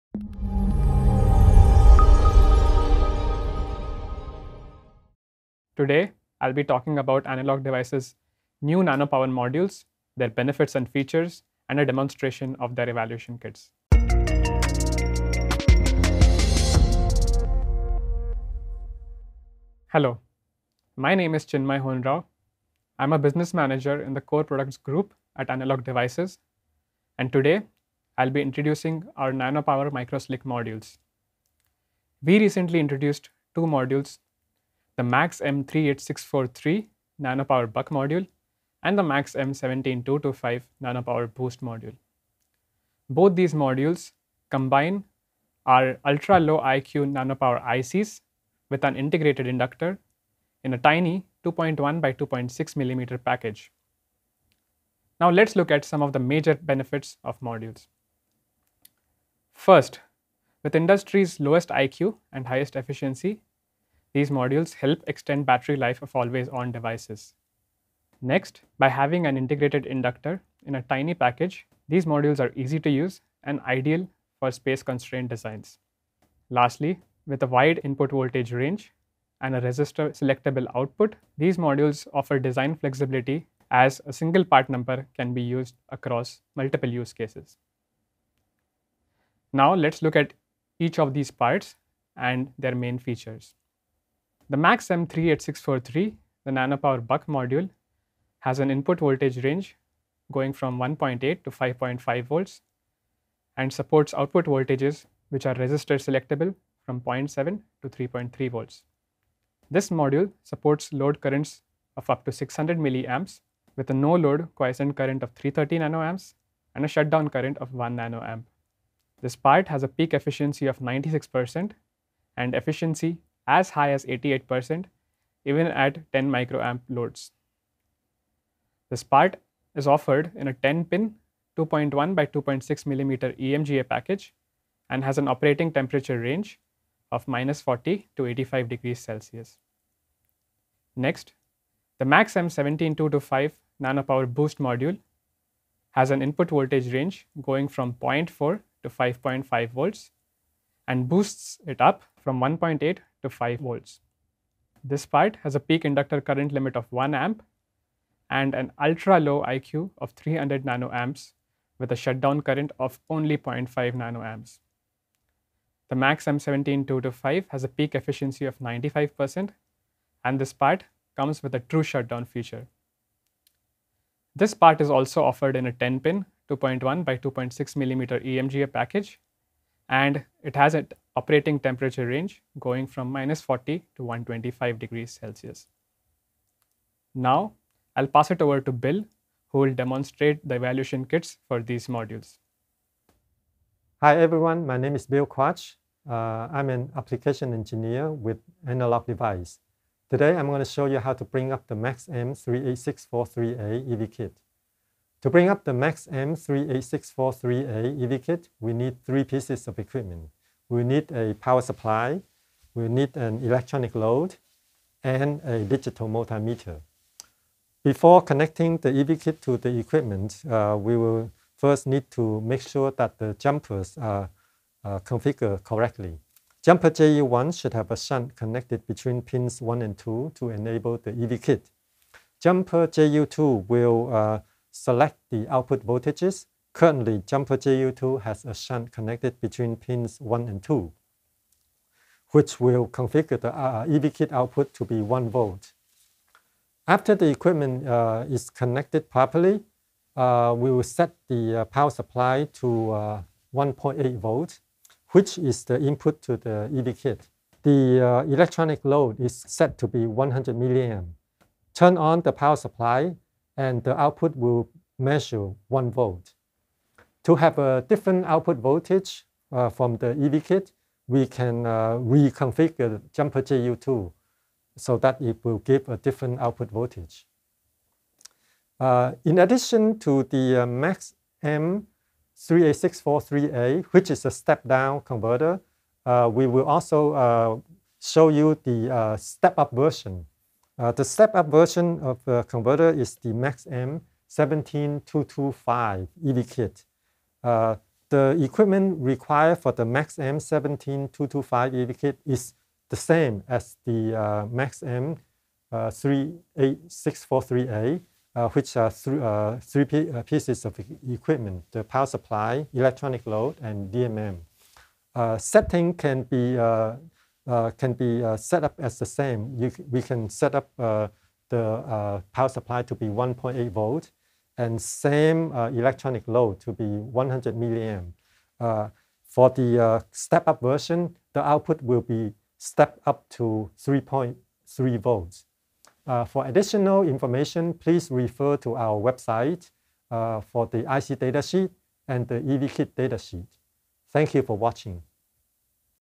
Extend Battery Life in Space-Constrained Applications with ADI's nanoPower Modules
Find out more about MAXM38643 and MAXM17225 - power modules that feature the lowest quiescent current compared to competitive solutions and discover how to get started with their EV Kits.